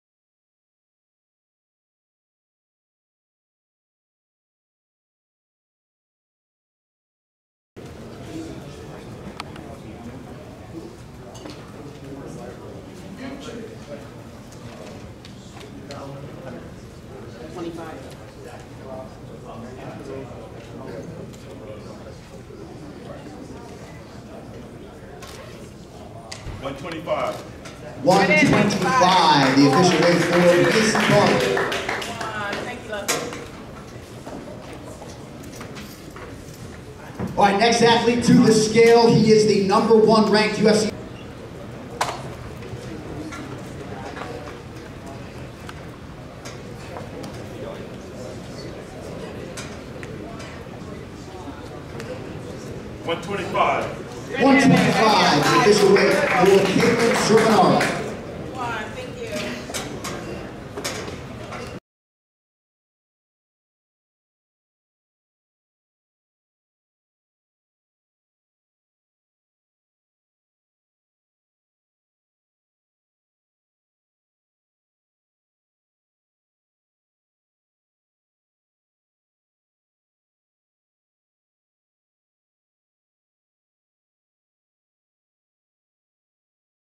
Maycee Barber & Katlyn Cerminara weigh in for UFC 299
Katlyn Chookagian and Maycee Barber weigh in for UFC 299